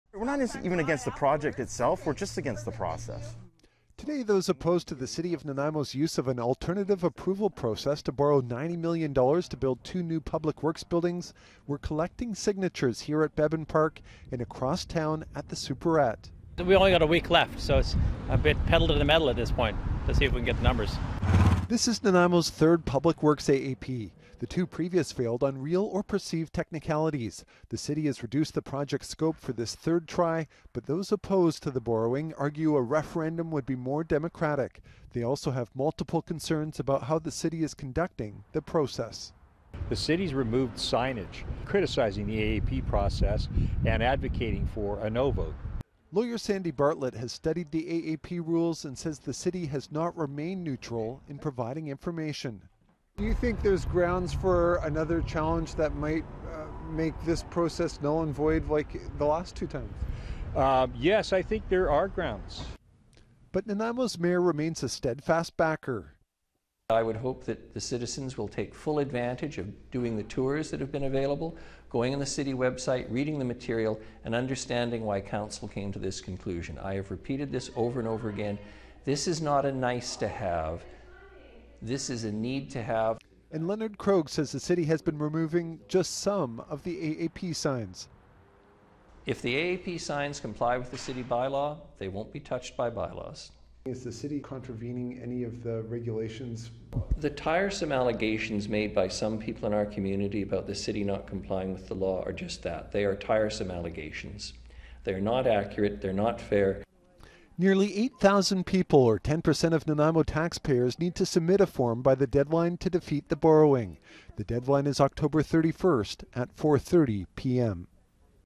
AAP opposers accuse City of Nanaimo of stealing signs
There's one week left in a controversial Alternative Approval Process (AAP) in Nanaimo along with new allegations.

Those opposed to the City of Nanaimo borrowing $90 million to build two new public works buildings say the city has been breaking the rules.